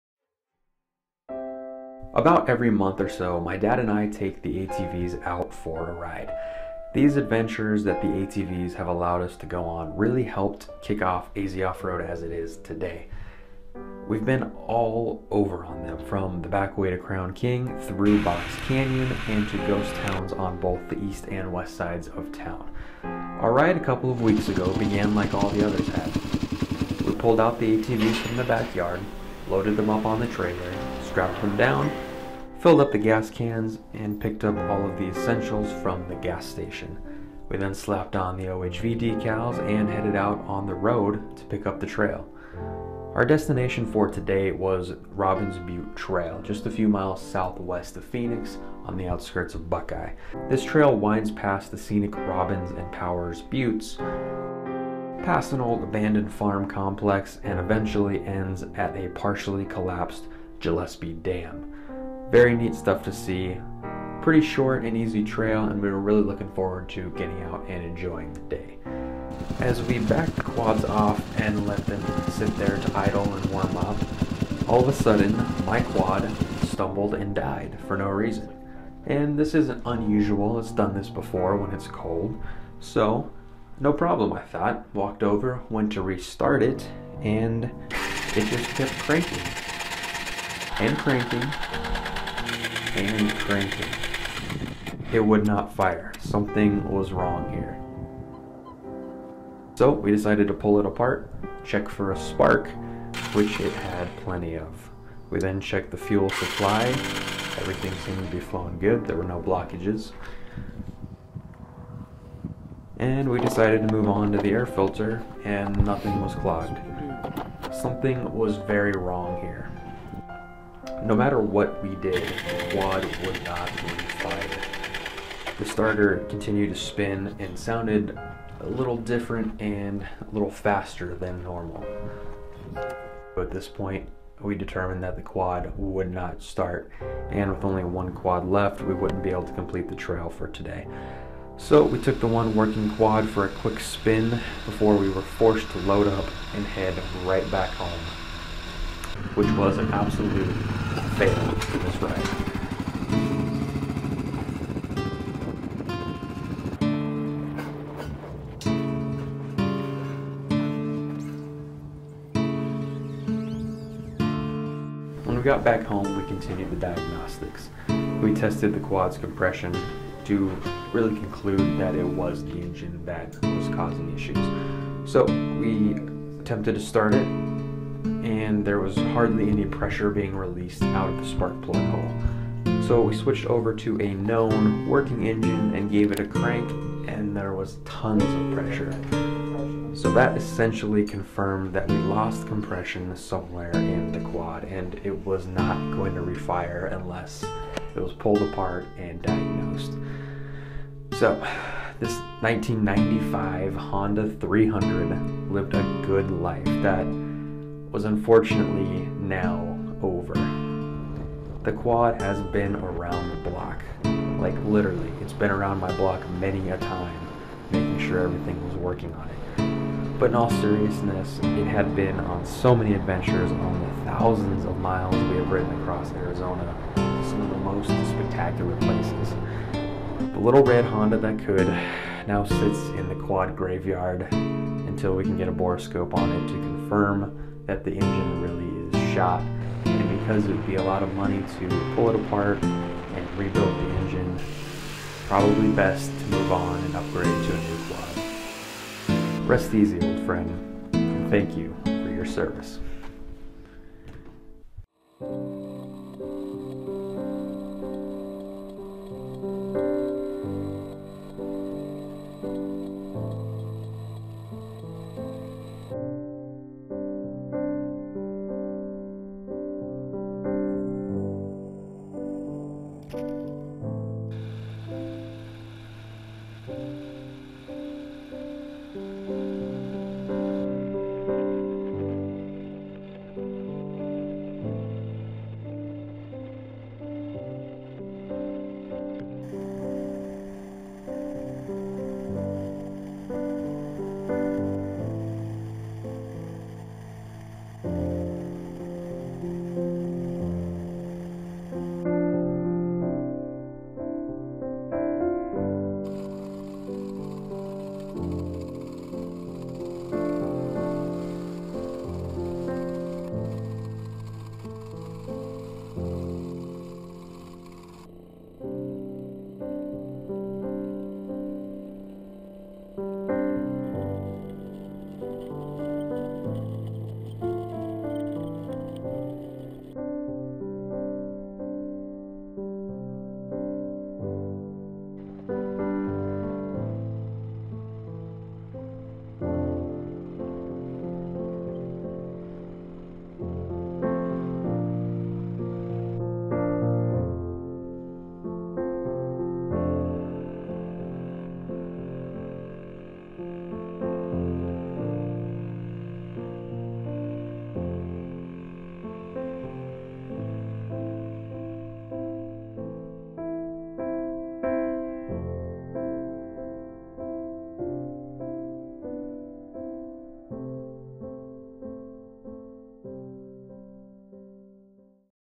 Killing the Honda 300
After 8 years of riding thousands of miles across Arizona, our 1995 Honda 300 finally had enough. On an attempt to ride Robbins Butte Trail in Buckeye, the quad stalled and wouldn't restart. Numerous attempts to fix the quad were without success and we determined the engine lost compression.

The quad put in some serious work on the trail and will be missed. Rather than spend a lot on an engine repair, we'll likely put that money into a newer quad.